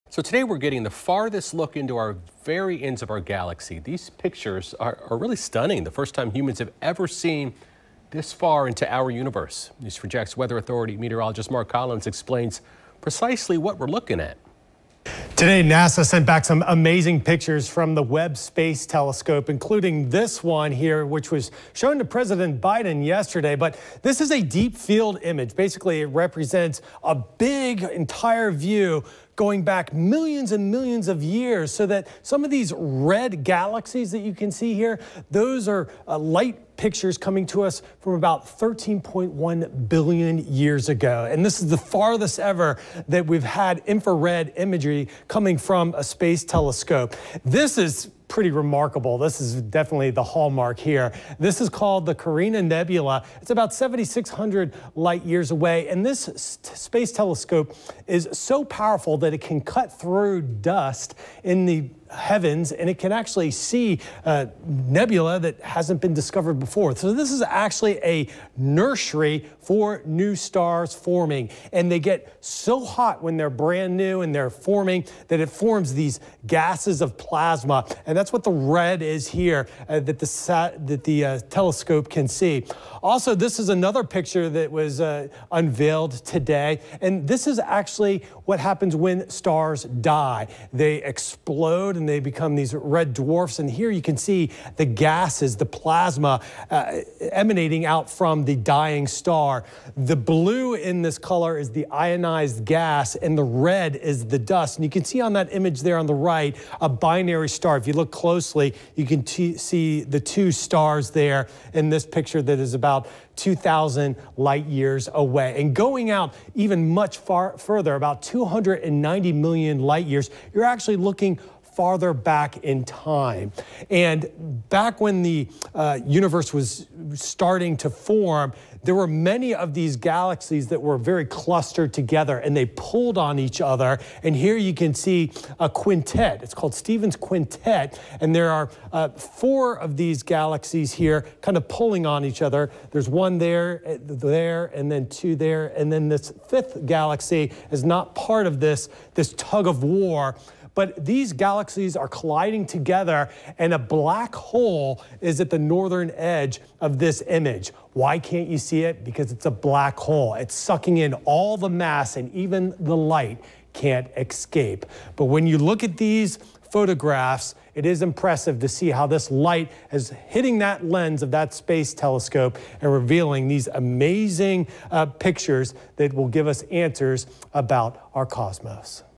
This is what the universe looked like over 13 billion years ago
A full set of pictures showing our universe in its earliest phase was released Tuesday by NASA.